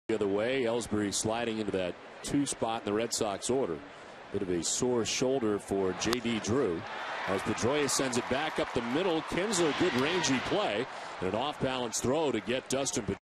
2009/06/06 Kinsler's strong throw
TEX@BOS: Kinsler makes a strong throw to get the out